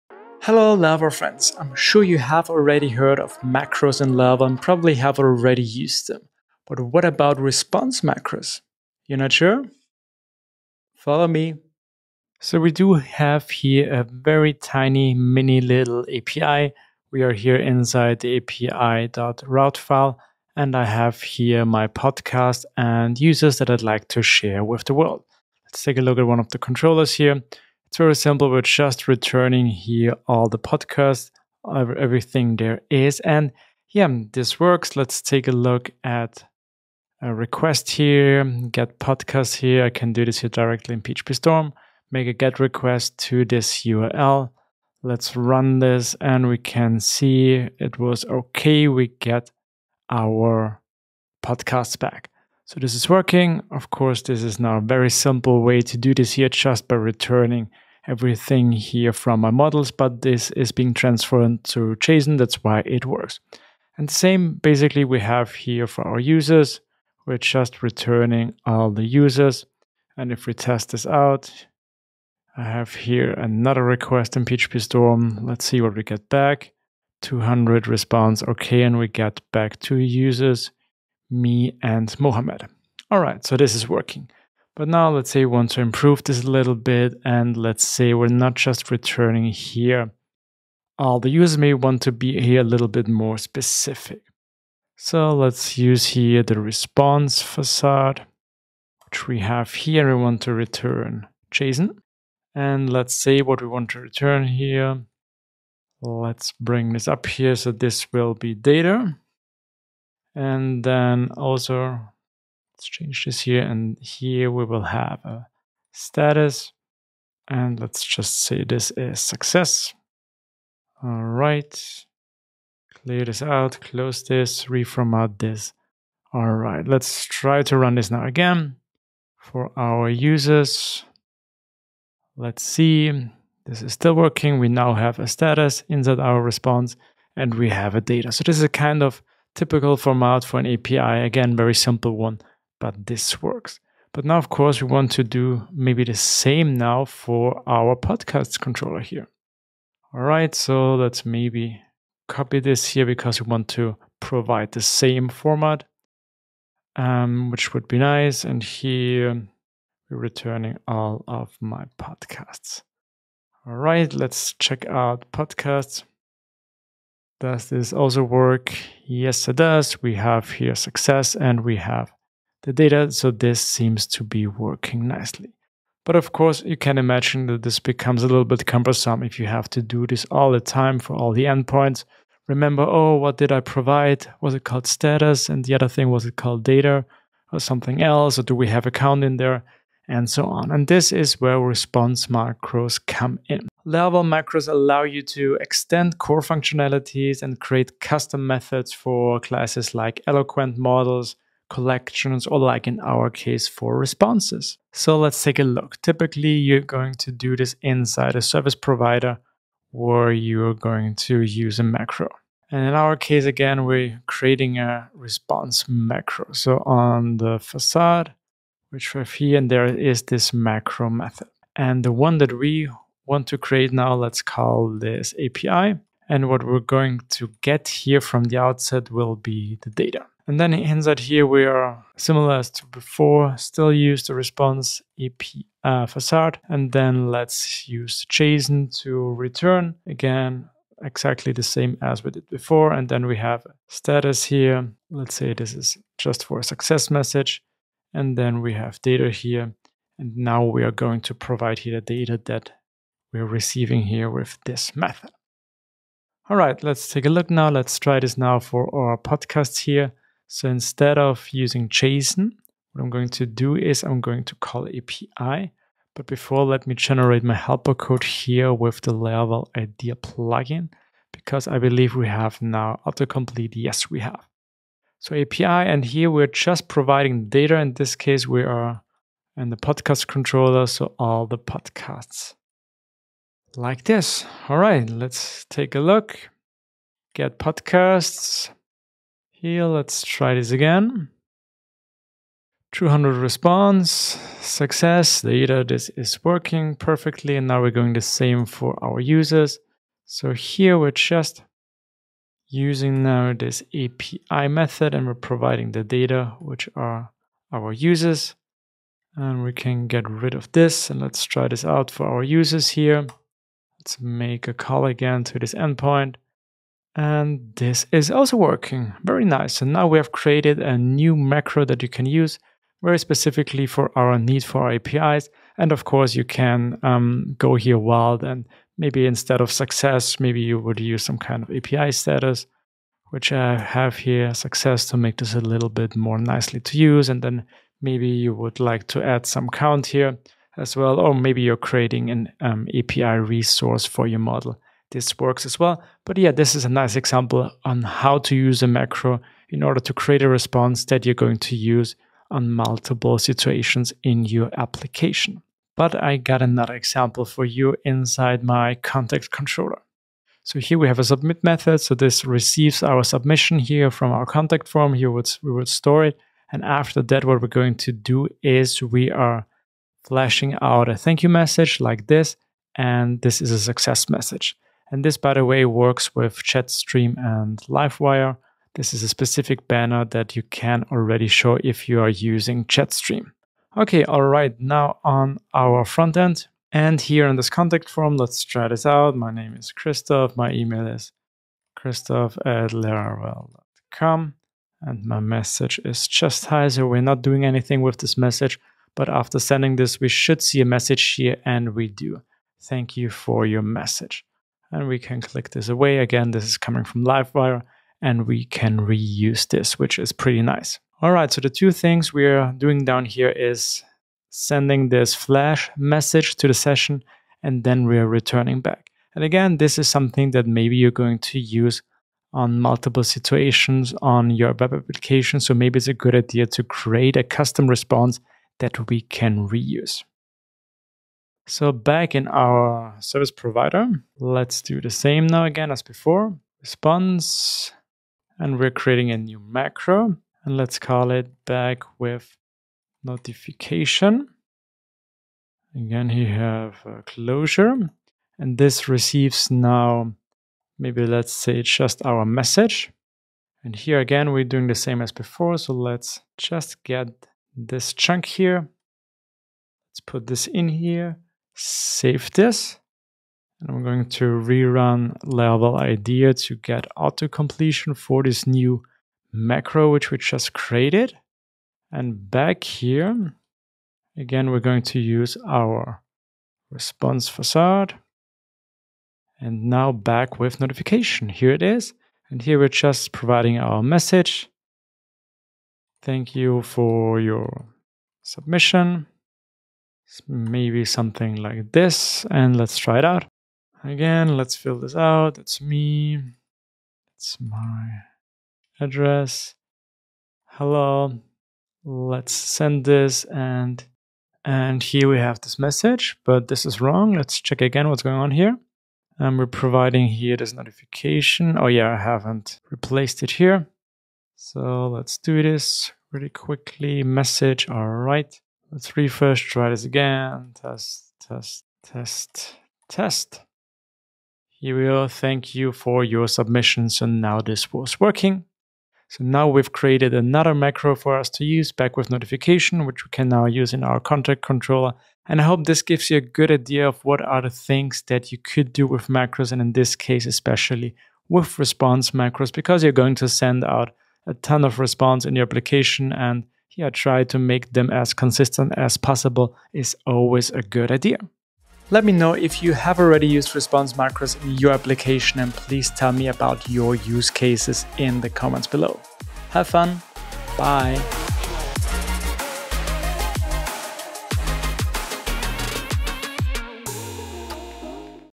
Laravel Gems - Response Macros 💎
Dive into "Laravel Gems," a series dedicated to unearthing hidden treasures of Laravel functionalities. Enhance your skill set as we illuminate overlooked yet powerful features you might not know existed.

➡️ Today: Response Macros

00:00 Welcome
00:12 Api Response
05:55 Contact Response
09:11 Outro